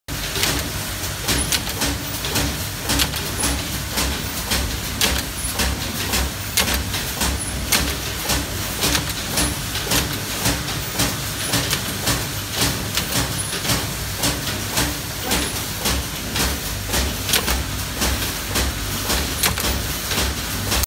HY-107N Semi-auto nylon zipper top stop machine
Top stop is to prevent slider come out of zipper, there's many different style of top stop, from the video is one type, use aluminum wire stamp on the end of zipper.